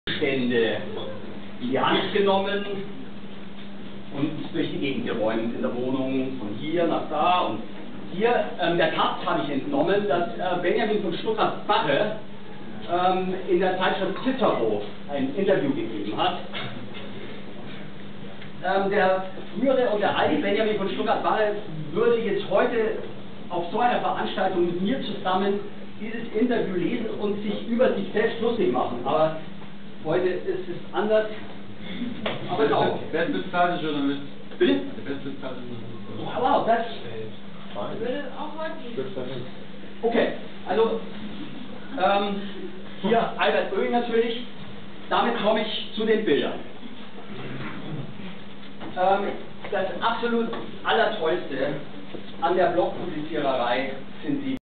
barre cicero blog ... DANKESGESANG
Samstag, 21. Juni 2008, 22 Uhr
Oranienstraße 189, Berlin, bei Anne Neukamp
es waren alle herzlich eingeladen
KLAGE feierte Abschied